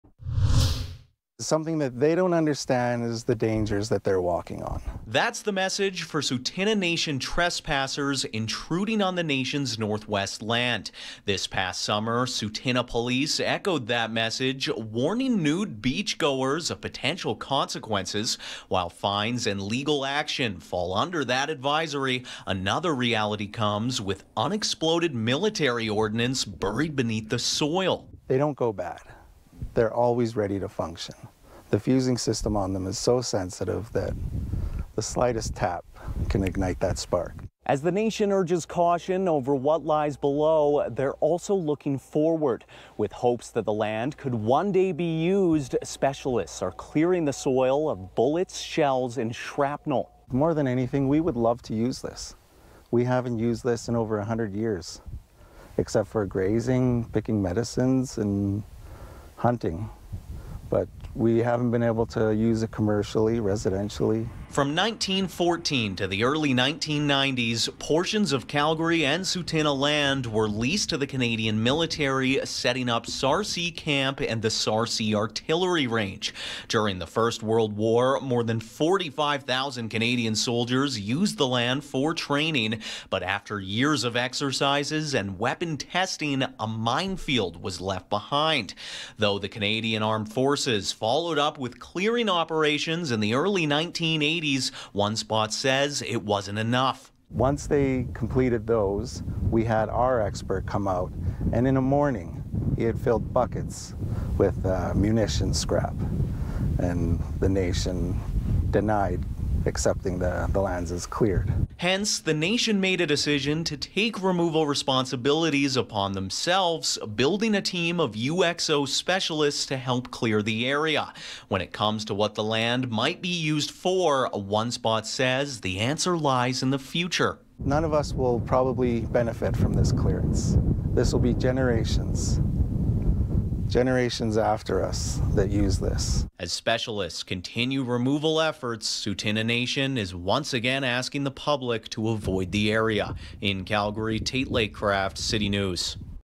Tsuut’ina Nation clearing lands of unexploded ordnance
After decades of military drills and weapon testing on Tsuut’ina Nation land, specialists are clearing soil of unexploded military ordnance. With more on Tsuut’ina's efforts, Tate Laycraft reports.